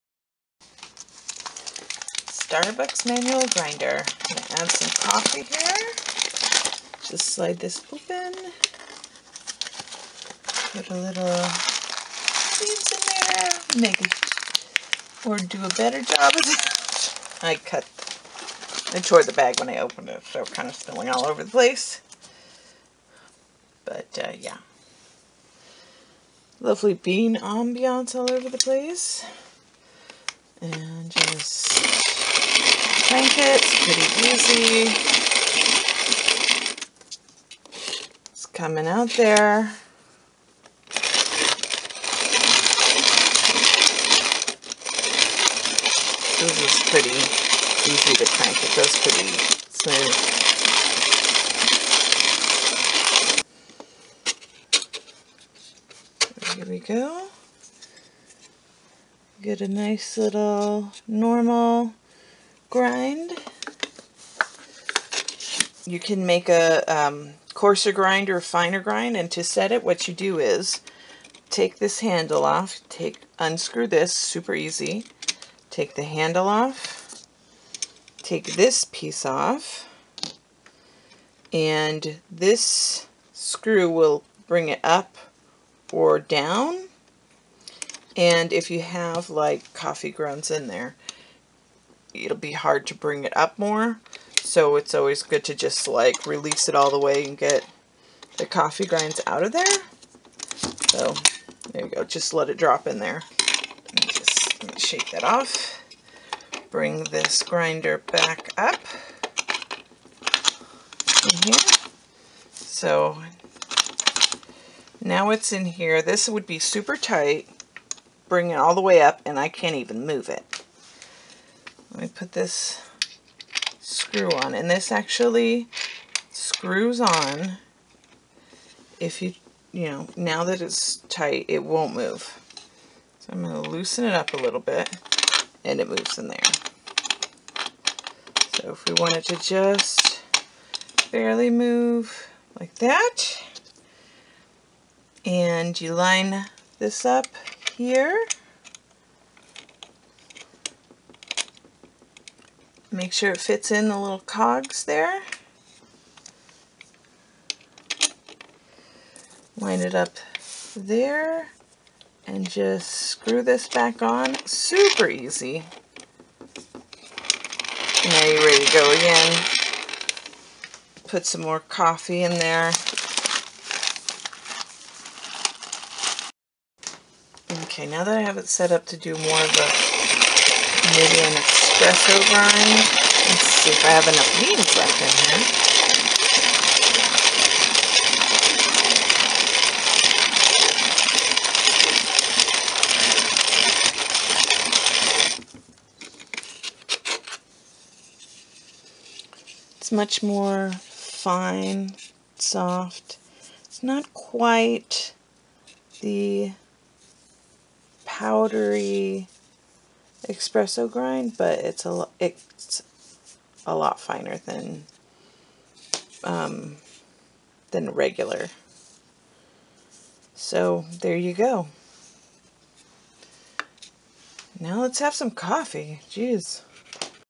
Starbuck's manual coffee grinder
How it works and raising and lowering the grinder for finer or coarser coffee grounds.